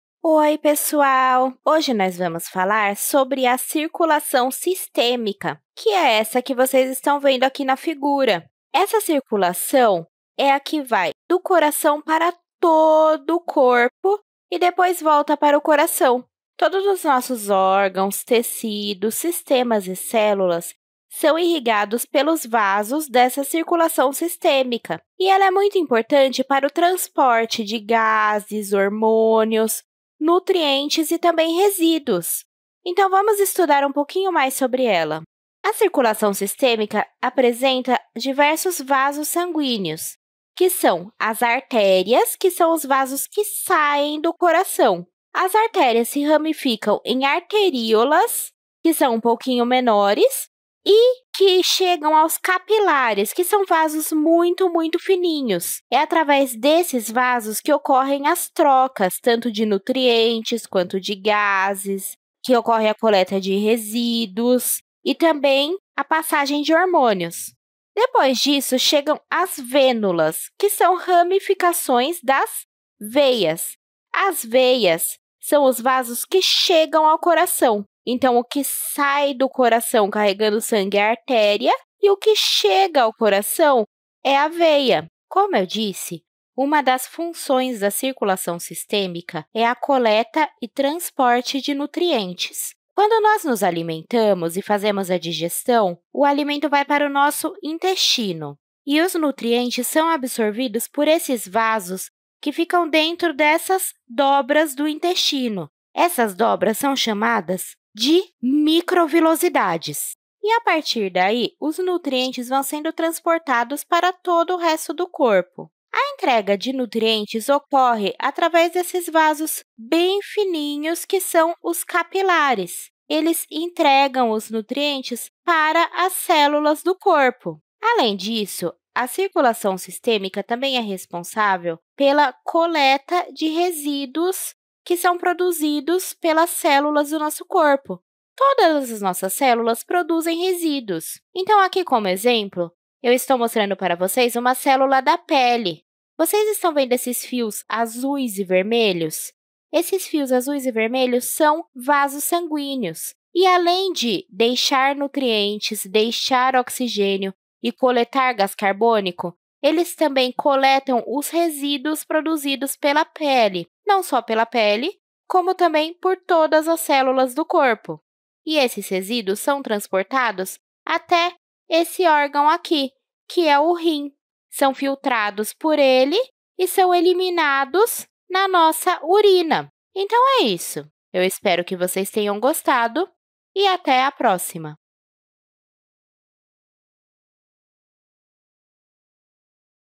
A circulação sistêmica, a entrega de nutrientes e retirada de resíduos
Nesta videoaula apresentamos a circulação sistêmica, mostrando os caminhos percorridos pelo sangue, a entrega de nutrientes e a coleta e eliminação dos resíduos produzidos durante o metabolismo.